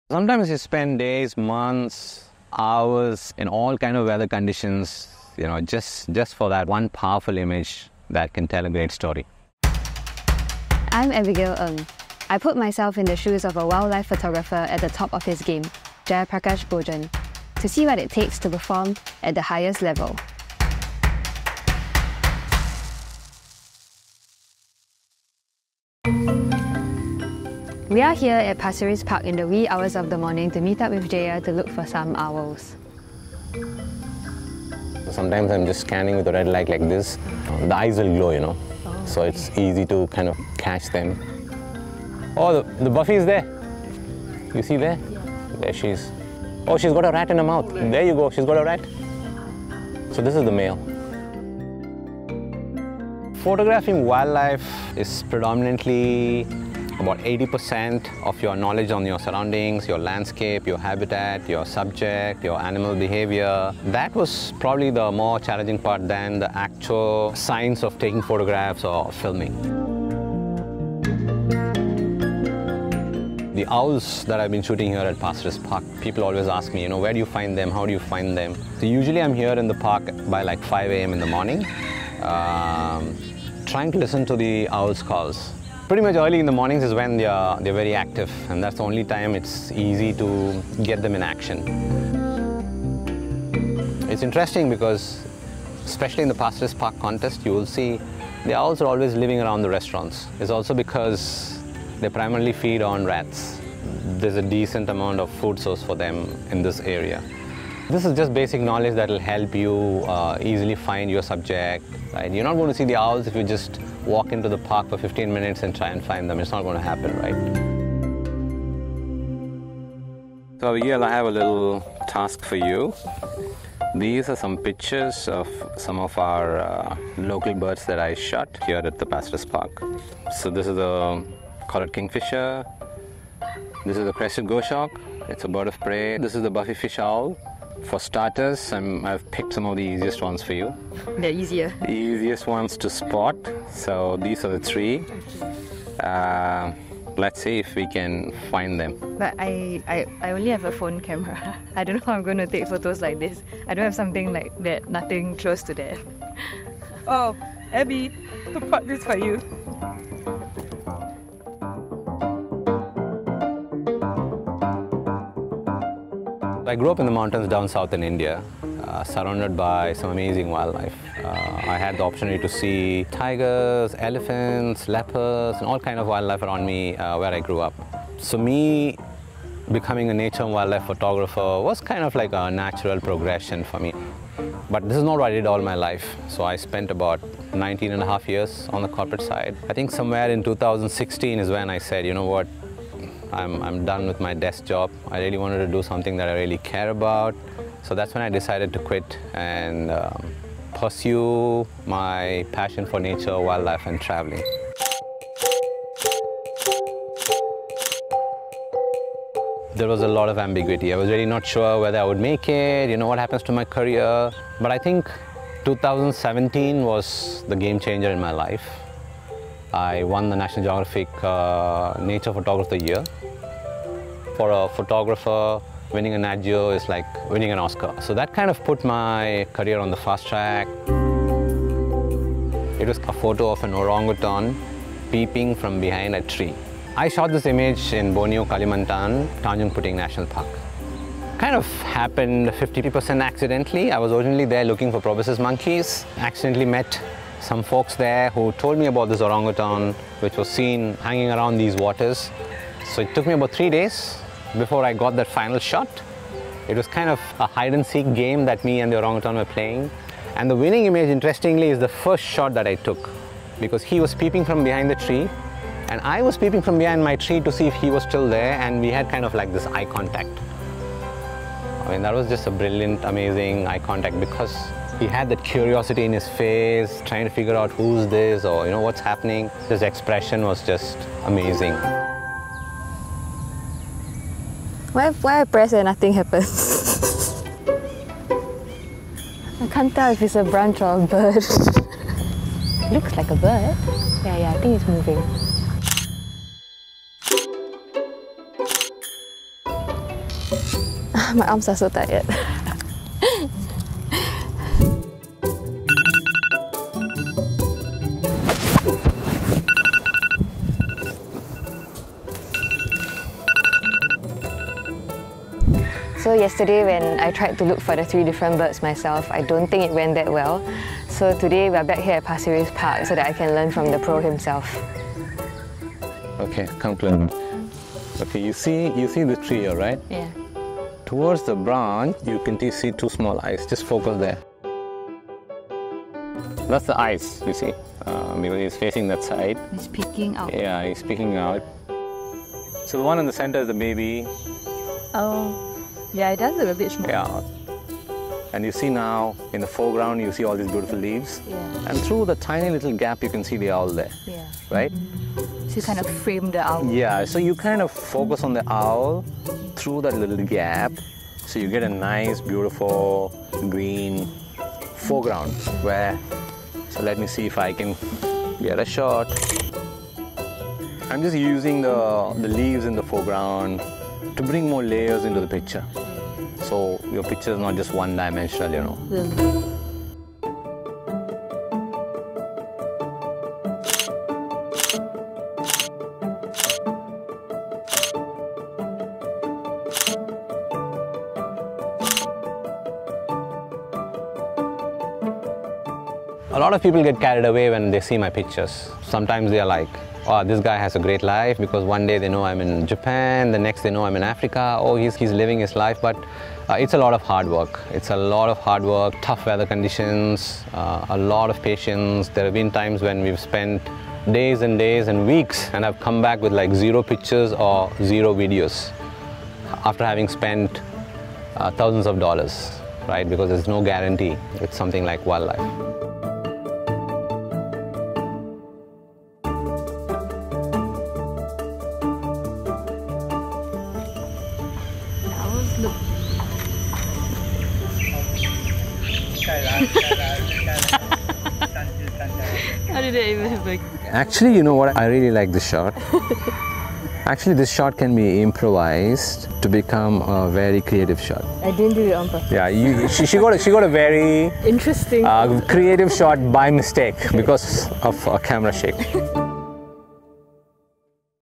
What it takes to be an award-winning wildlife photographer
What does it take to become a top wildlife photographer? Jayaprakash Bojan, 2017 National Geographic Nature Photographer of the Year, tells CNA's Abigail Ng his story and shares a few tips on spotting owls at Pasir Ris Park in Singapore.